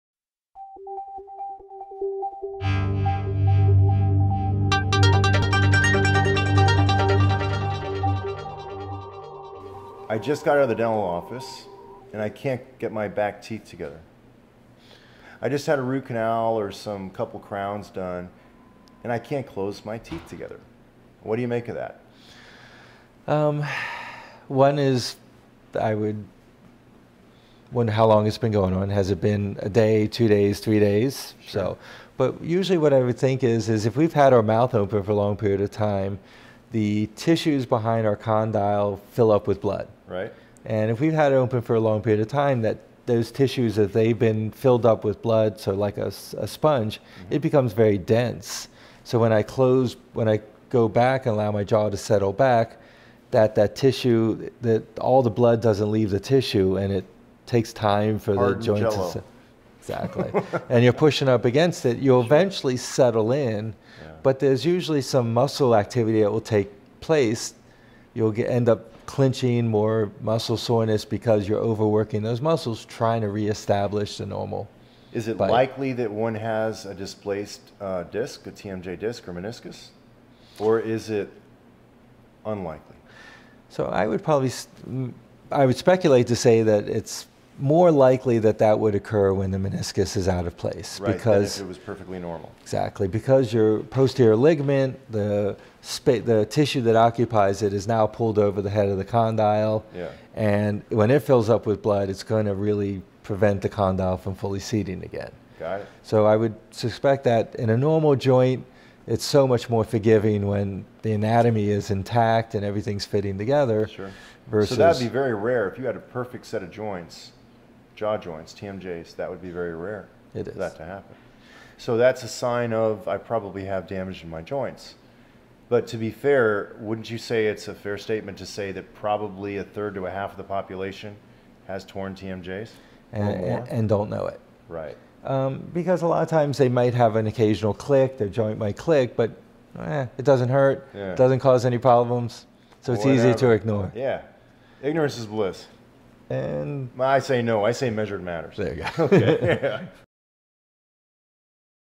Jaw won't close?! TMJ Open Lock
TMJ surgeon Dr. Ed Zebovitz of Bowie, MD explains to Dr. Nick what an open lock could mean for the patient and their dentist.